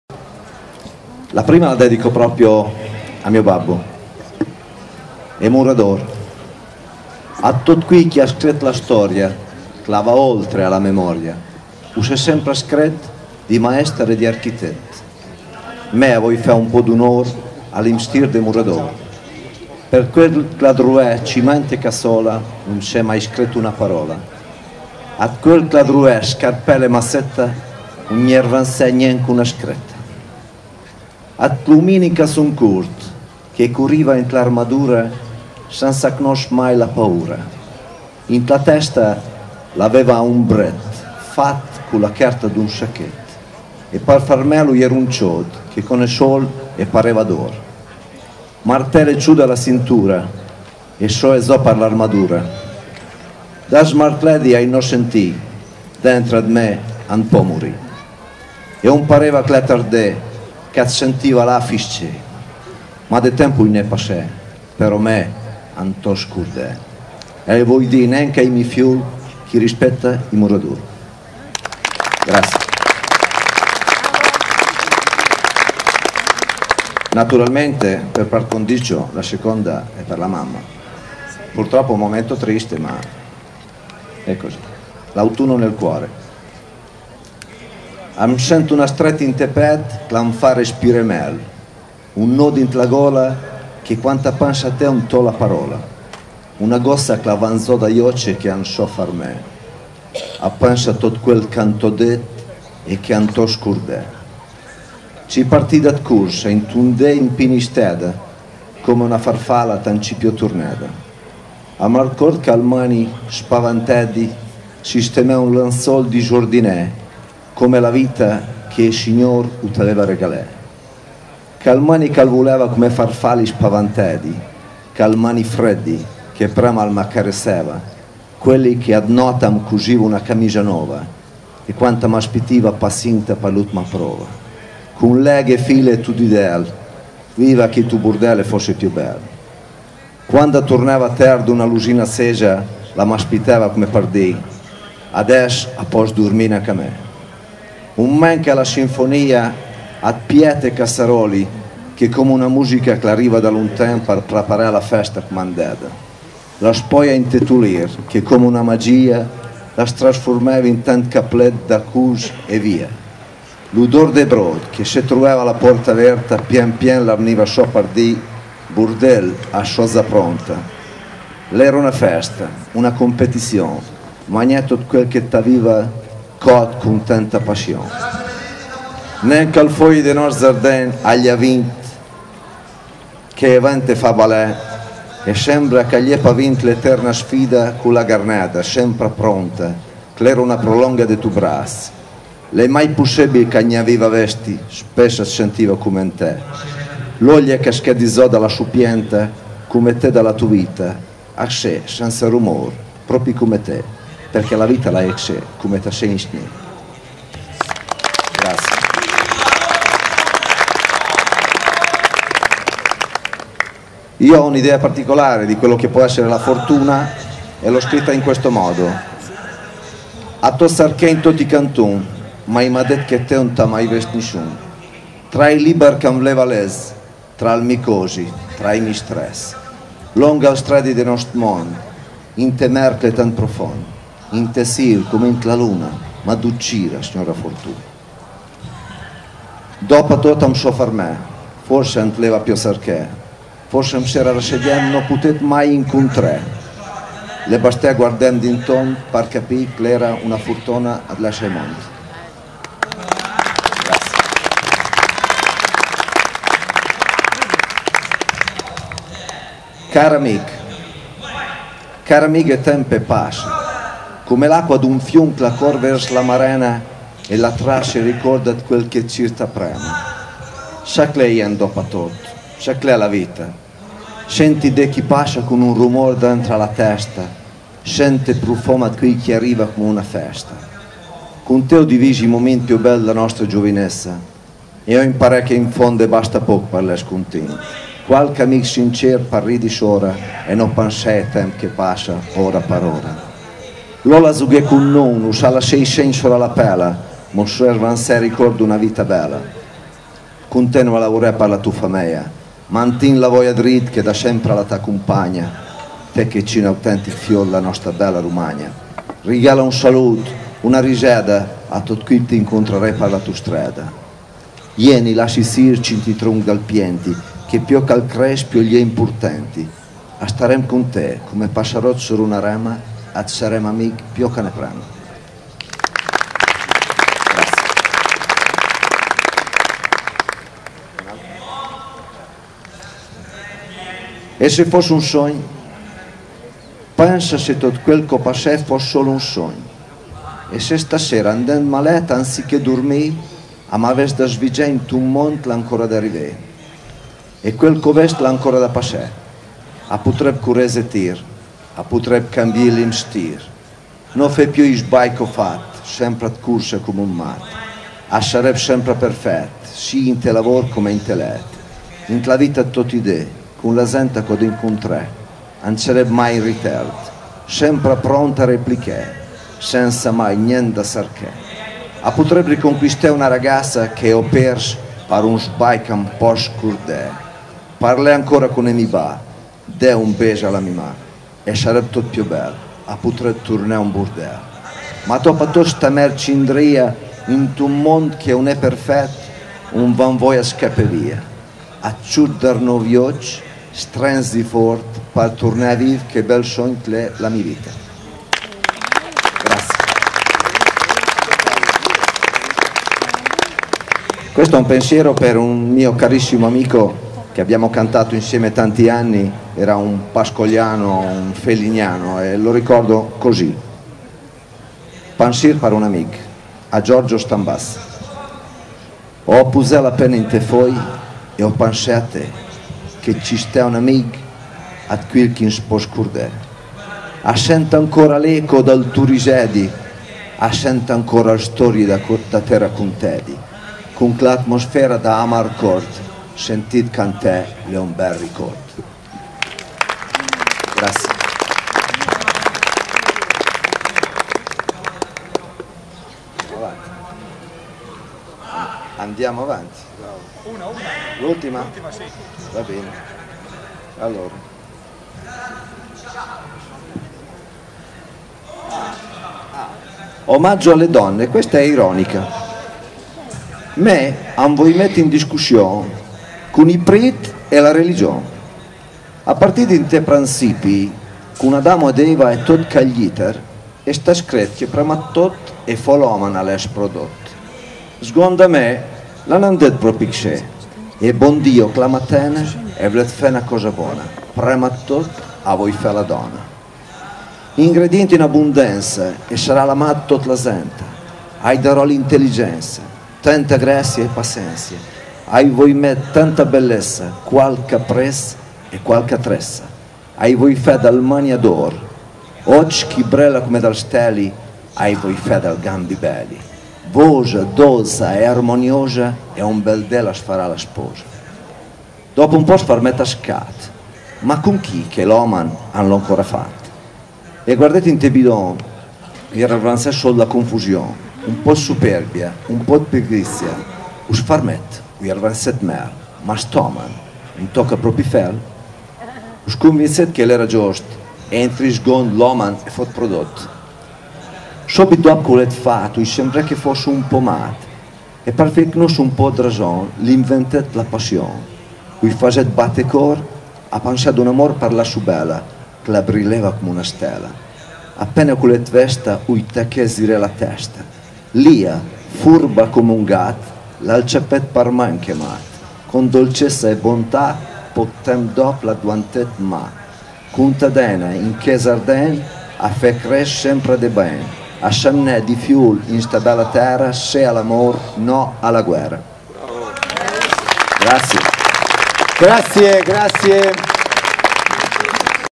Te ad chi sit e' fiol? 2013 - Valter Salvi
Un momento della quinta festa del dialetto romagnolo "Te ad chi sit e' fiol?" alla Rocca Malatestiana di Cesena, domenica 7 luglio 2013 dalle 16,00 fino a sera.

Ritorno alle radici, DIALETTO LINGUA VIVA.